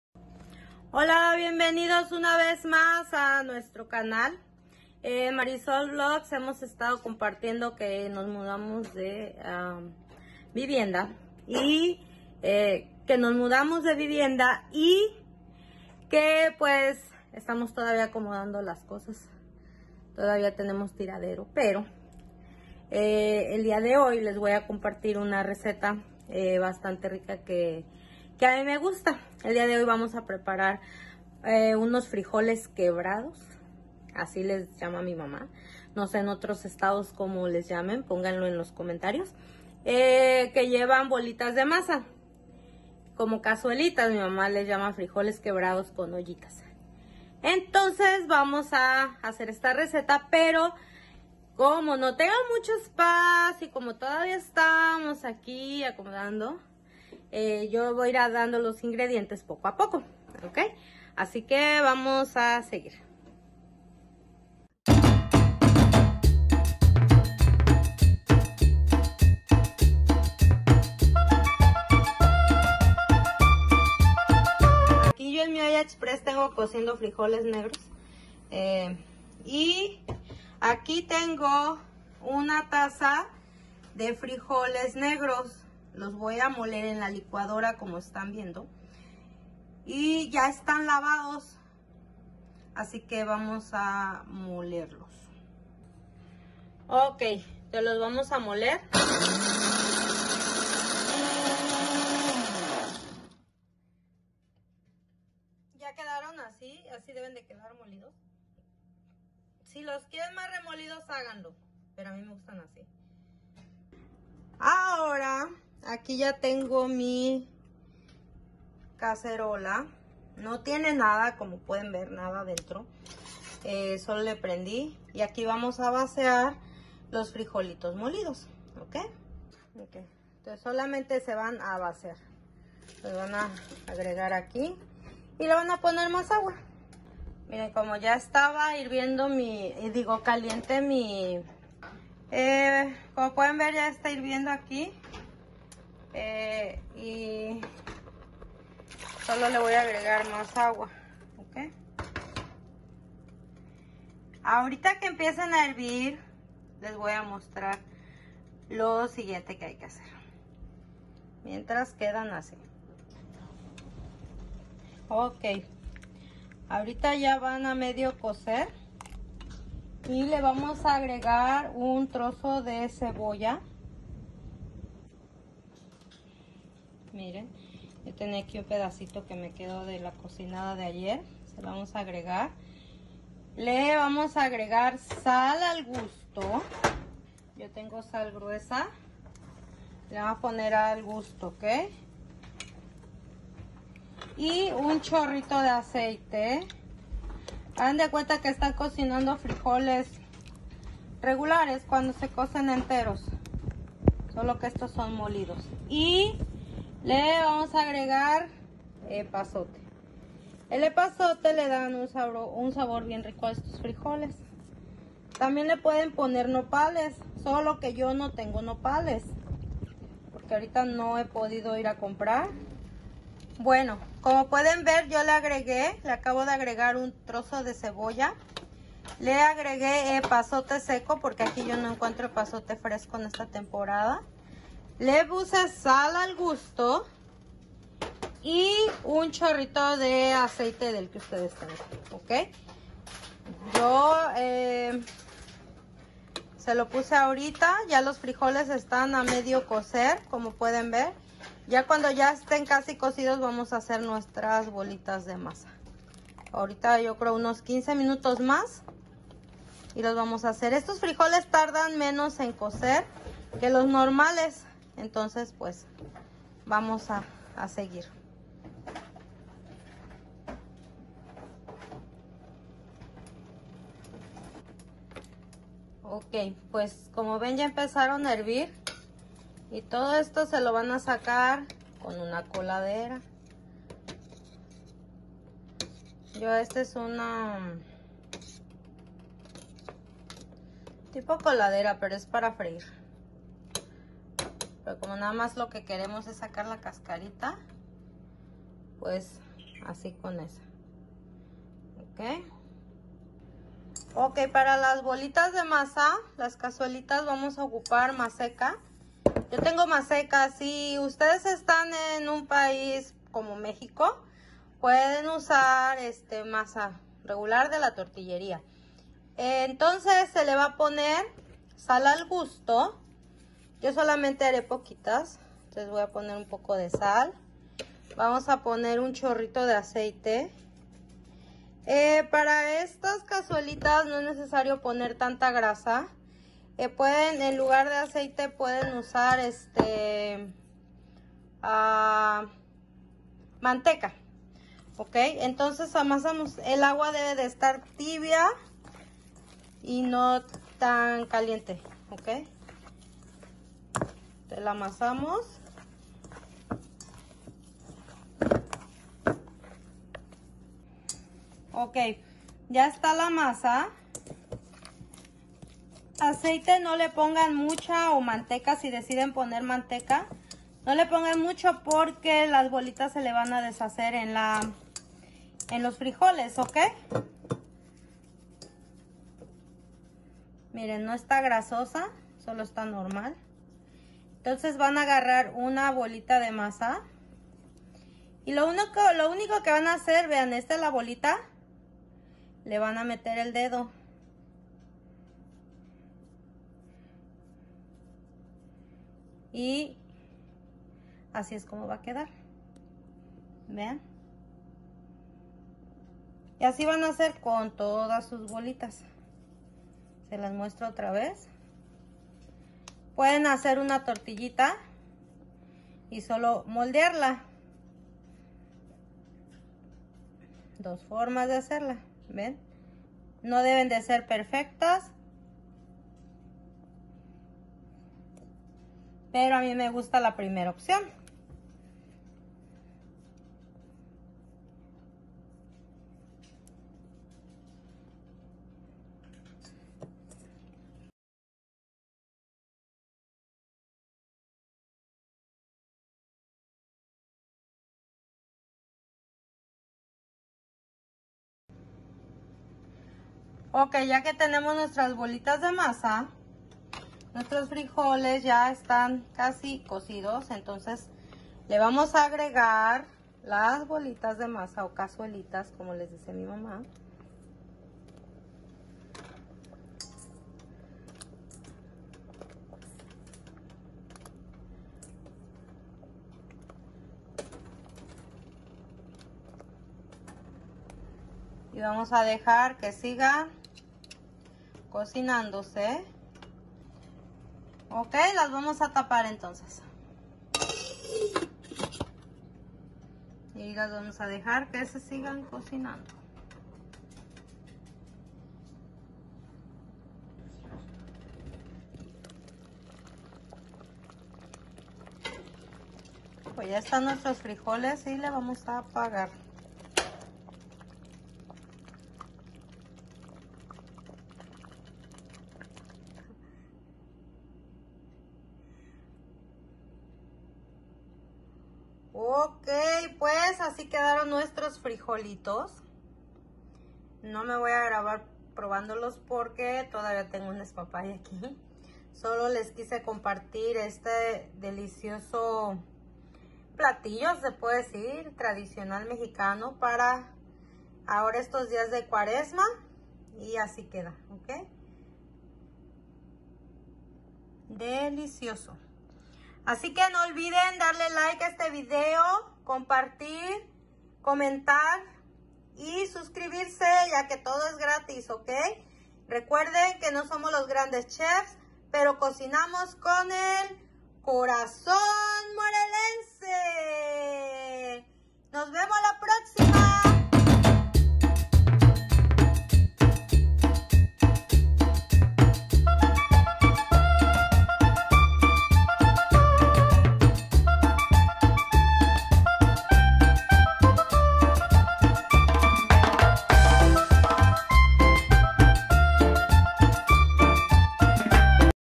Frijoles 🧅Quebrados (con cazuelitas)🥘
hola a todos ya ando por aca otra vez , y es que ha sido dificil grabar con toda esta mudanza y la casa es un relajo, pero no me rajo y aqui les traigo otra rica receta, disfrutenla